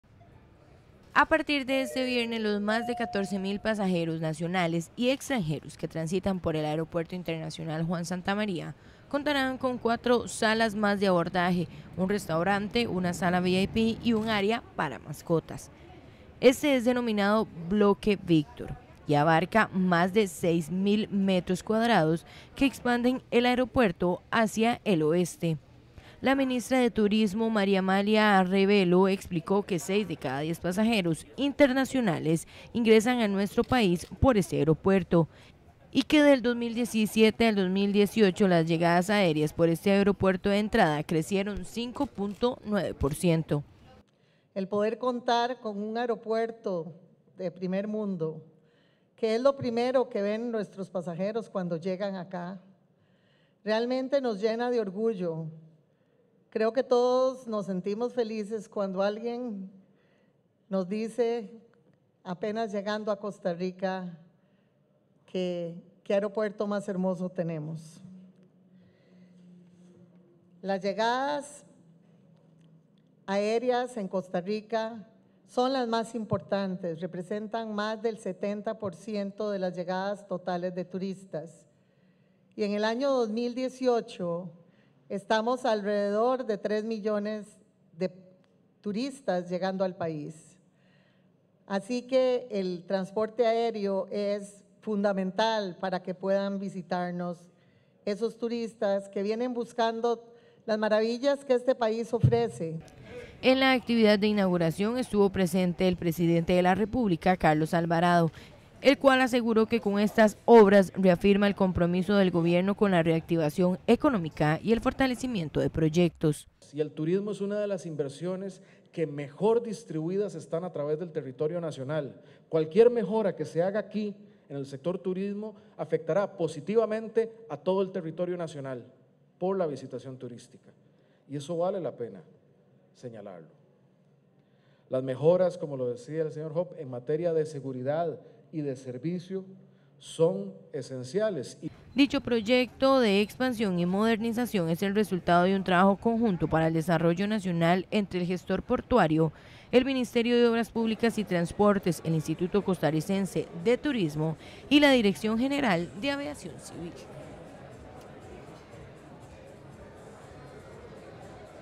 Habilitan nuevas salas de abordaje en el Santamaría
Este viernes se habilitaron 4 nuevas salas de abordaje en el Aeropuerto Internacional Juan Santamaría.